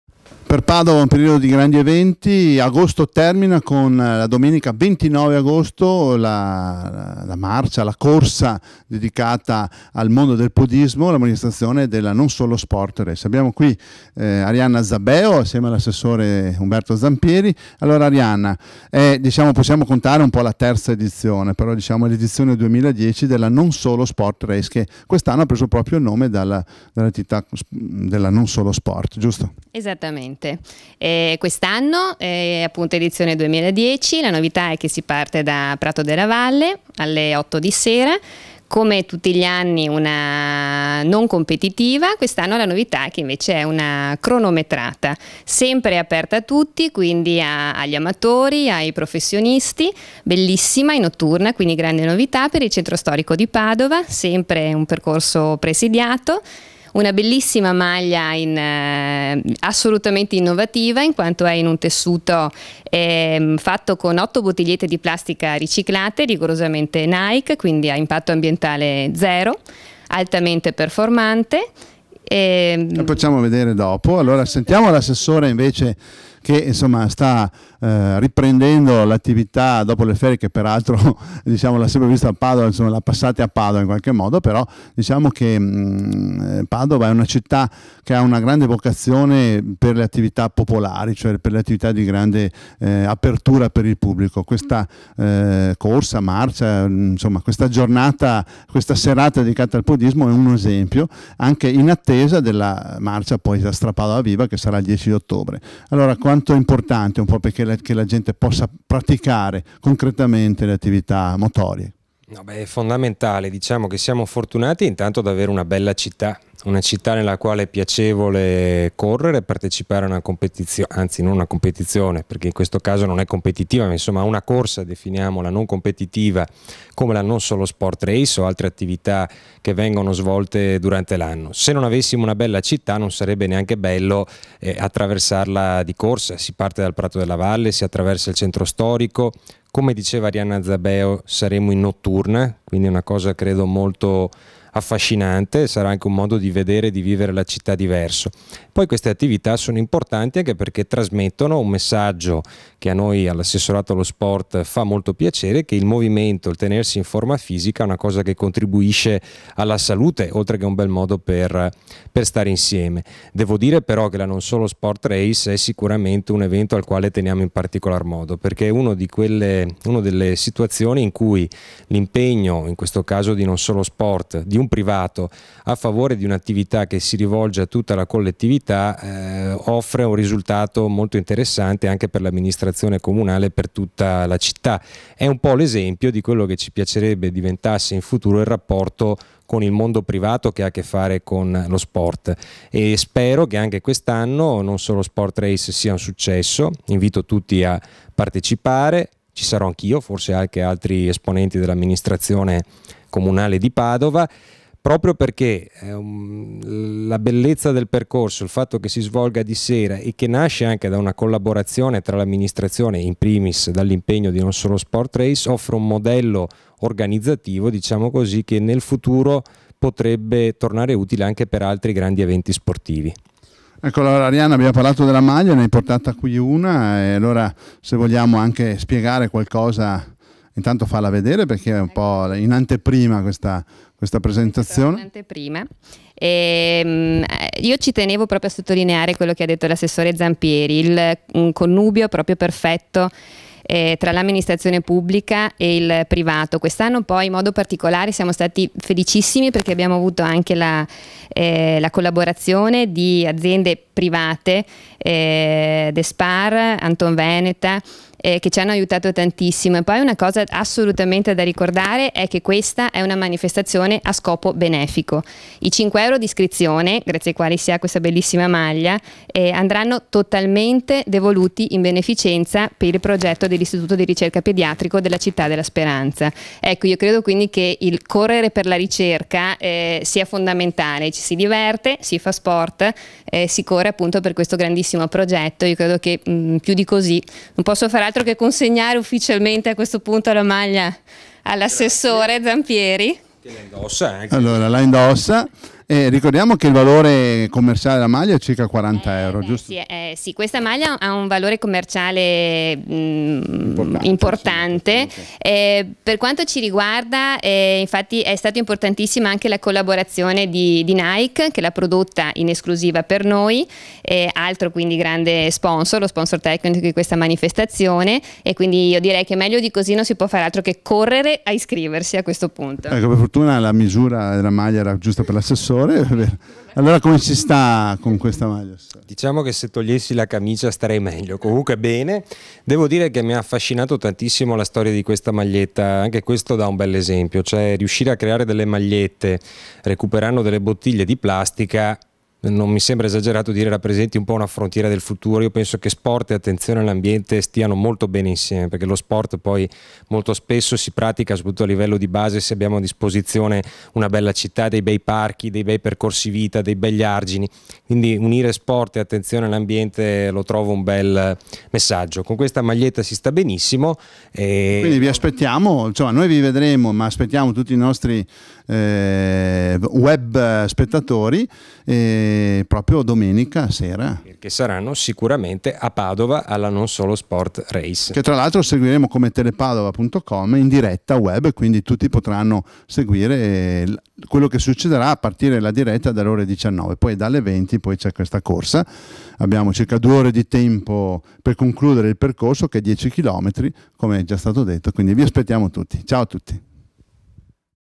NON SOLO SPORT RACE 2010
UMBERTO ZAMPIERI ASSESSORE ALLO SPORT DEL COMUNE DI PADOVA E ARIANNA ZABEO DELLA NON SOLO SPORT RACE PRESENTANO LA CORSA CHE SI TERRA' A PADOVA IL 29 AGOSTO 2010